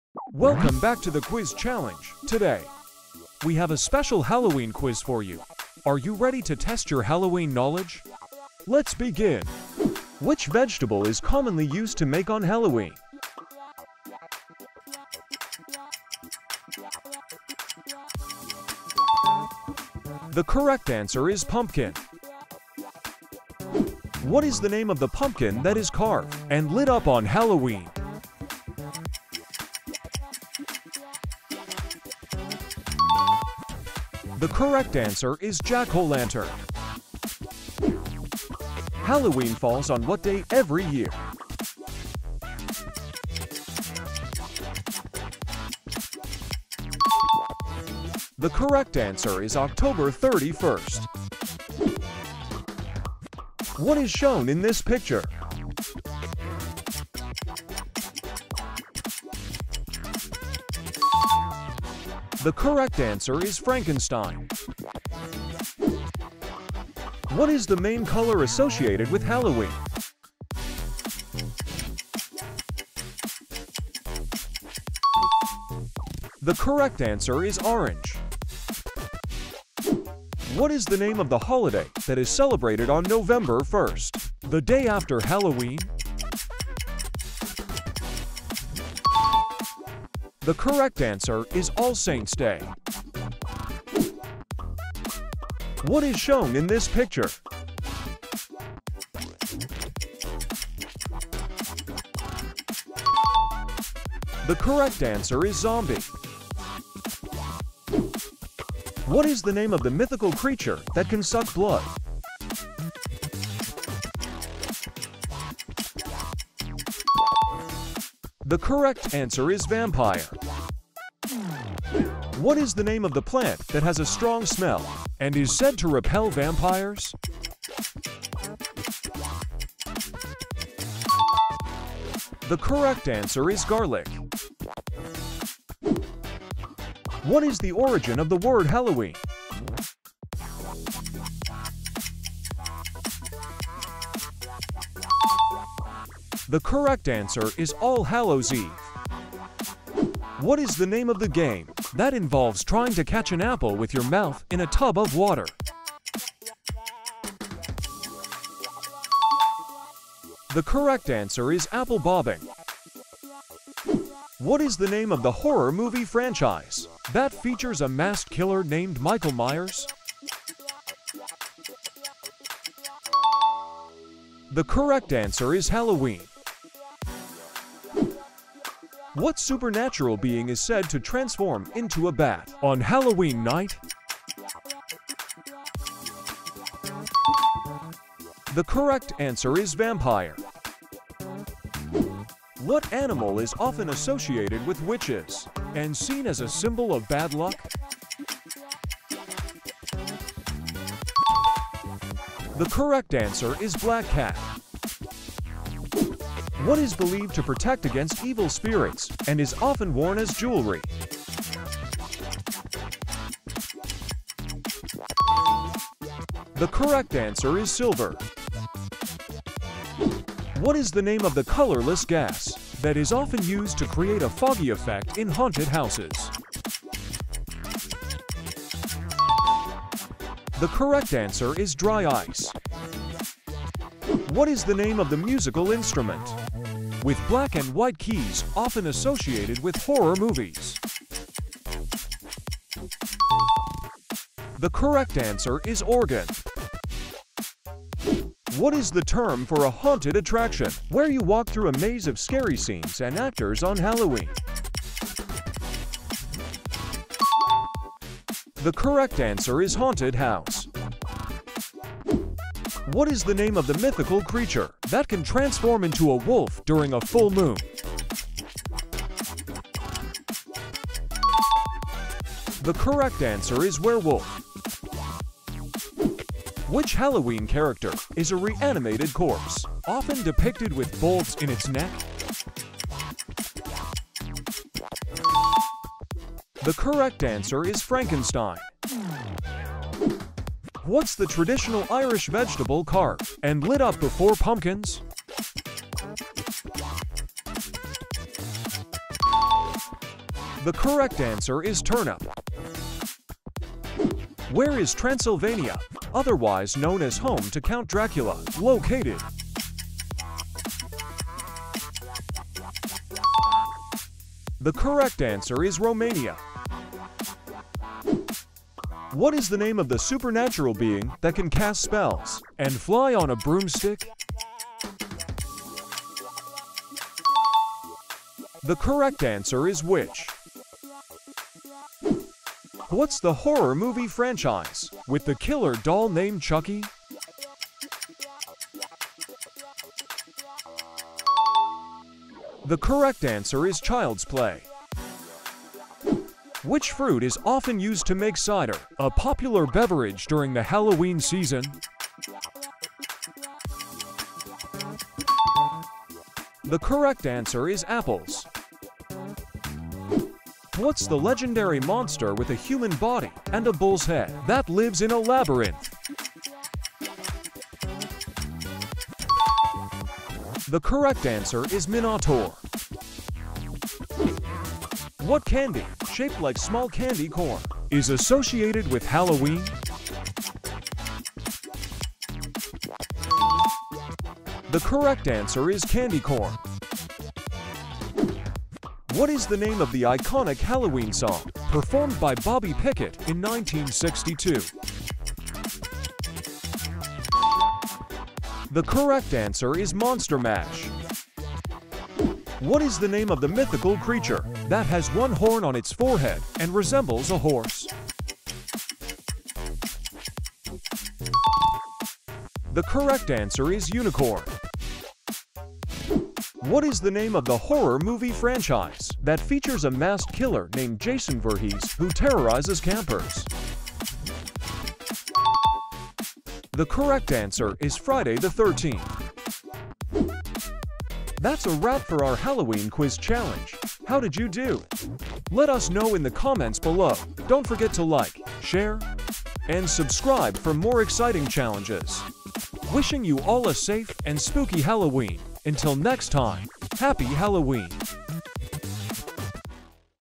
The Halloween Trivia Quiz: Test Your Spooky Knowledge!"
Welcome to "The Quiz Challenge!" – and today, we have a spine-tingling treat for you: our Halloween Quiz! Get ready to put your knowledge of all things Halloween to the test in this spooktacular edition of our popular quiz show.

In this video, our engaging host presents you with a series of Halloween-themed questions, each designed to challenge your Halloween expertise. From pumpkin carving traditions to legendary monsters, supernatural beings, and iconic horror movies, this quiz covers it all. Can you answer all the questions correctly and prove yourself as the ultimate Halloween aficionado?

As you navigate through the quiz, you'll encounter intriguing multiple-choice questions with options that'll keep you on your toes. Do you know your Jack-o'-lanterns from your Chucky dolls or your witches from your werewolves? Test your Halloween knowledge and discover just how much you know about this spooktacular holiday.

Whether you're a Halloween enthusiast or just looking for a fun and educational way to get into the Halloween spirit, this video has you covered.

That's a wrap for our Halloween Quiz Challenge! How did you do? Share your results and celebrate your Halloween expertise with us in the comments below. Wishing you all a safe and spooky Halloween! Until next time, Happy Halloween!